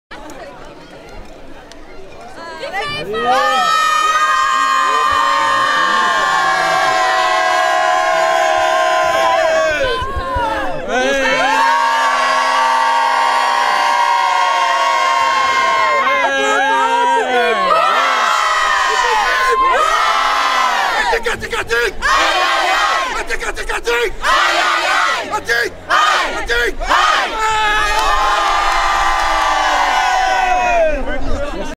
Petit aperçu en cut AFAO Course des Héros
Un petit aperçu de l'ambiance de la Course des Héros de Paris 2015 ! Merci à toutes nos héroïnes, à nos héros et à Bruno Rossi Productions d'avoir été présent ! Suite des images à venir !